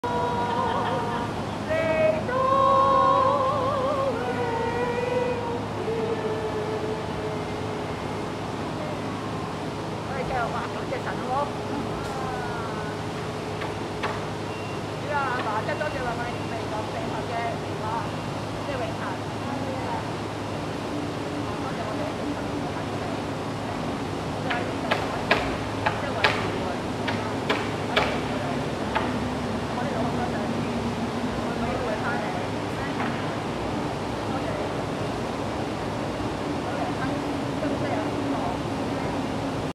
20200414_Coriander Studio_ Daniela Lam
Grandma had Christian worship outdoor